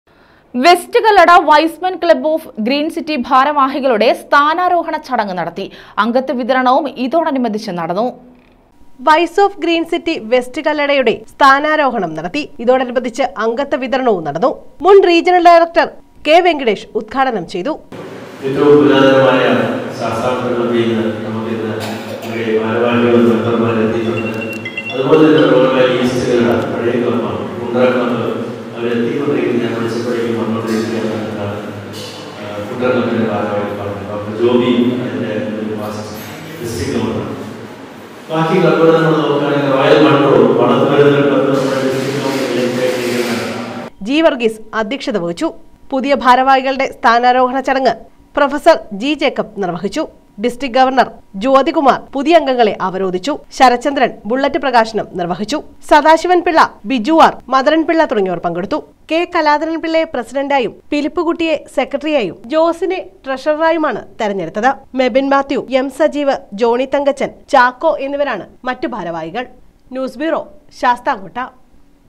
വെസ്റ്റ് കല്ലട വൈസ്‌മെന്‍ ക്ലബ്ബ് ഓഫ് ഗ്രീന്‍സിറ്റി ഭാരവാഹികളുടെ സ്ഥാനാരോഹണ ചടങ്ങ് നടത്തി.
വെസ്റ്റ് കല്ലട വൈസ്‌മെന്‍ ക്ലബ്ബ് ഓഫ് ഗ്രീന്‍സിറ്റി ഭാരവാഹികളുടെ സ്ഥാനാരോഹണ ചടങ്ങ് നടത്തി. അംഗത്വവിതരണവും ഇതോടനുബന്ധിച്ച് നടന്നു...